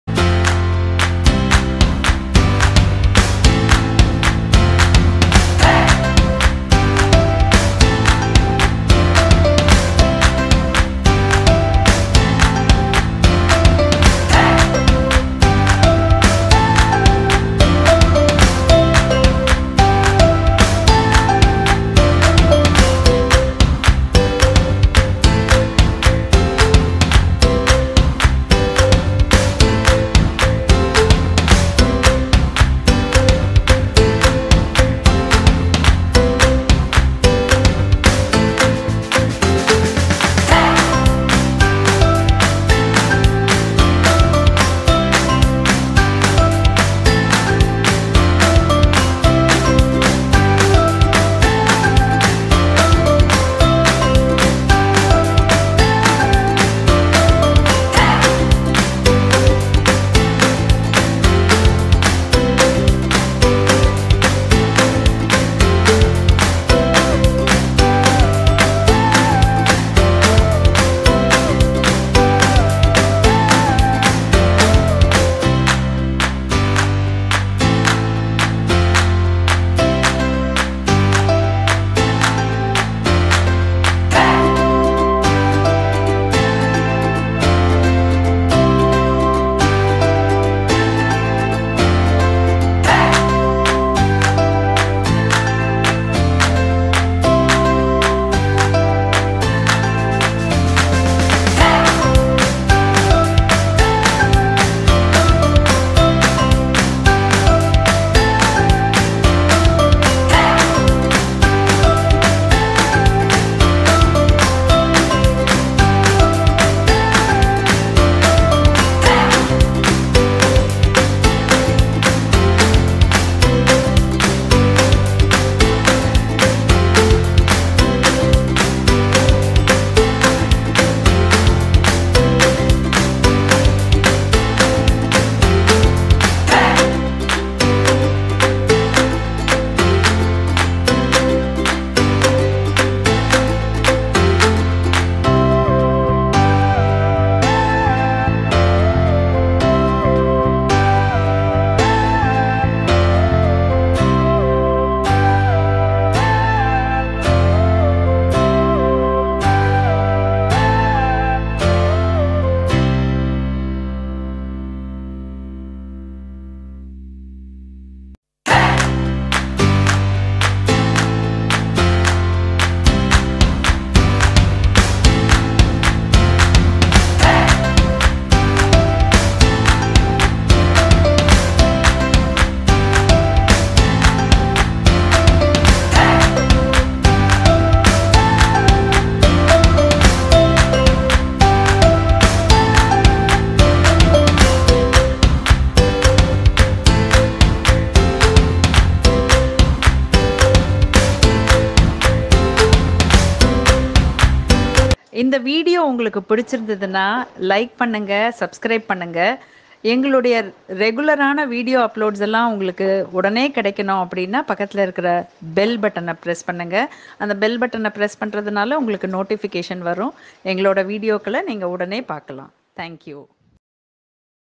மழை நேரத்தில் மொறு மொறுன்னு மசாலா பொரி || Masala Pori
Kids Special!!

Just enjoy this awesome Snack on a rainy day!

Ingredients used:
Roasted Bengal Gram (Pottu kadalai)
Groundnut
Puffed Rice
Turmeric Powder
Asafoetida
Salt to Taste
Coconut oil for Roasting
Curry Leaves